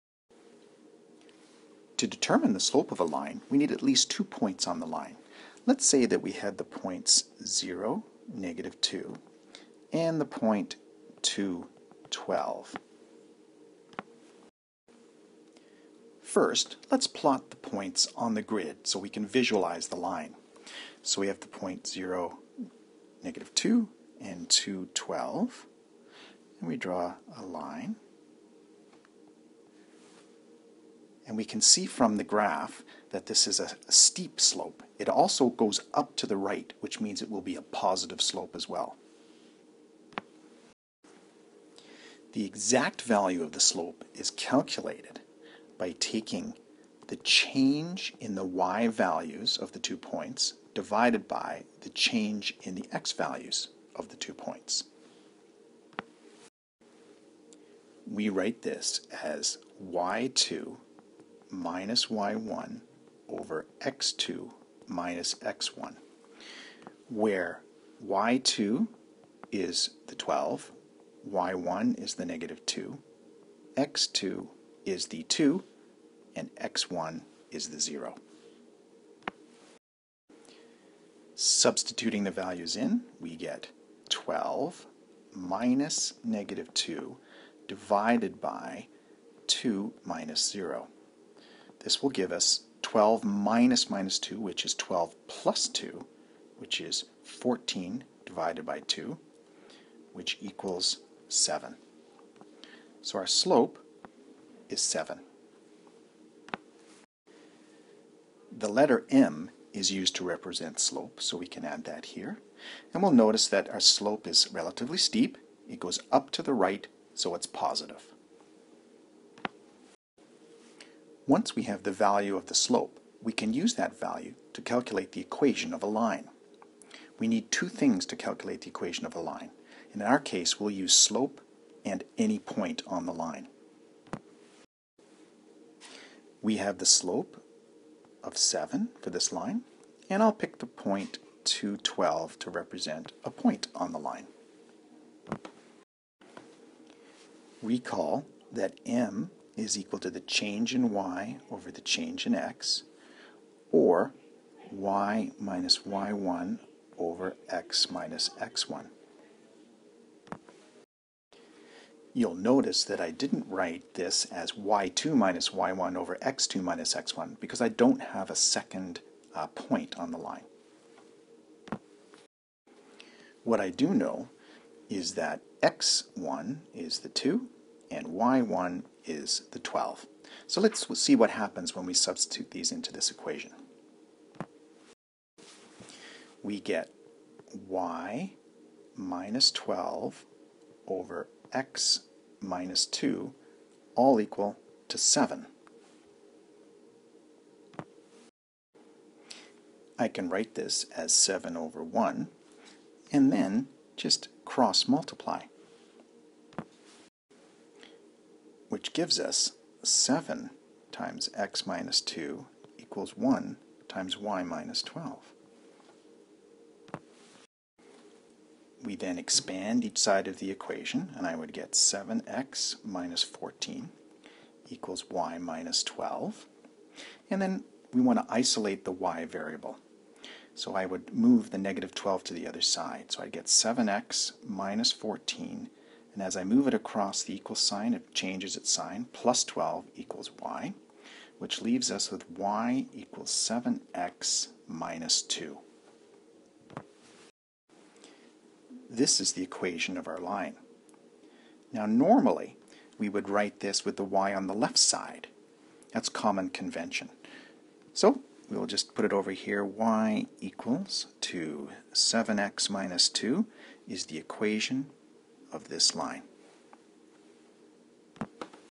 Finding the slope and the equation of a line
This video demonstrates how to find the slope and the equation of a line given any 2 points on the line.